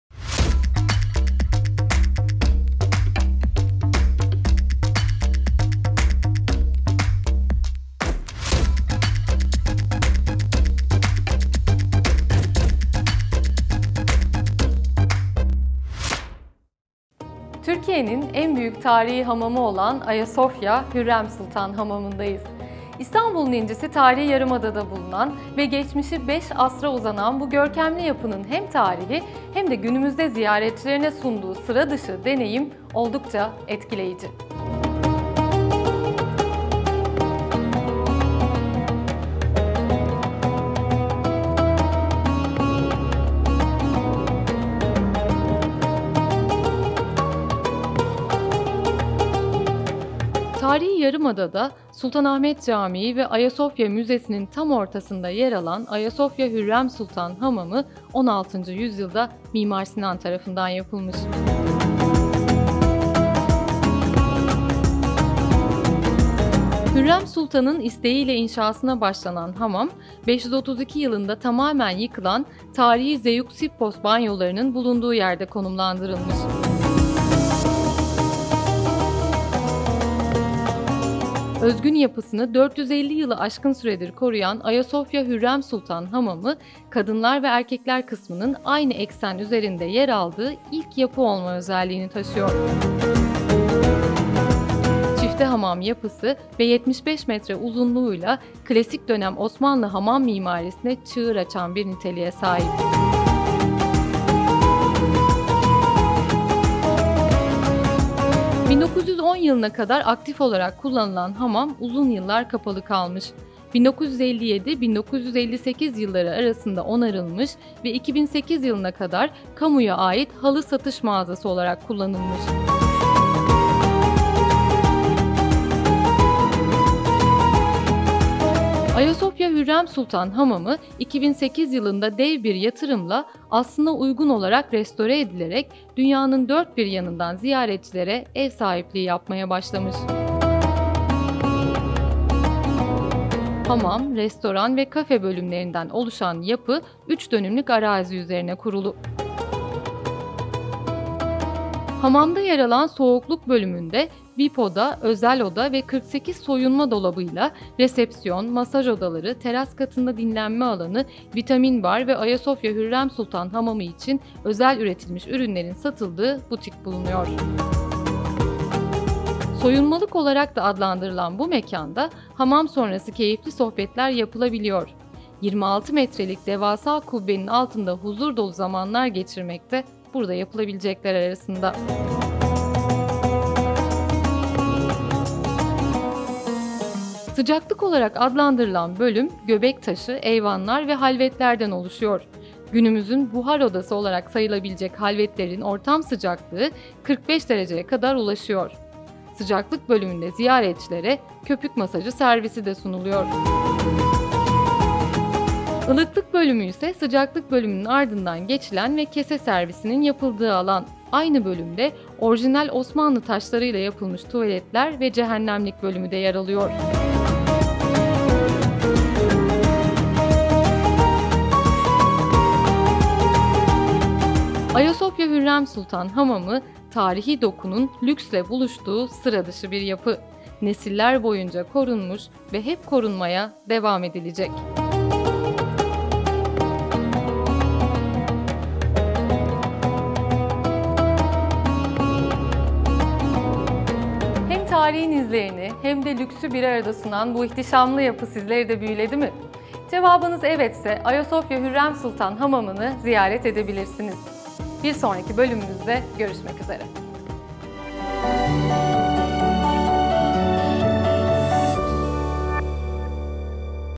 SIRA DIŞI YAPILAR 21. BÖLÜM - AYASOYFA HÜRREM SULTAN HAMAMI
Sıra Dışı Yapılar’ın bu bölümünde; Türkiye’nin en büyük tarihi hamamı olan ve özgün yapısını 450 yılı aşkın süredir koruyan Ayasofya Hürrem Sultan Hamamı’ndayız.

Tarihi dokunun estetikle buluştuğu bu sıra dışı yapının hikayesi şimdi KİPTAŞ Youtube kanalımızda!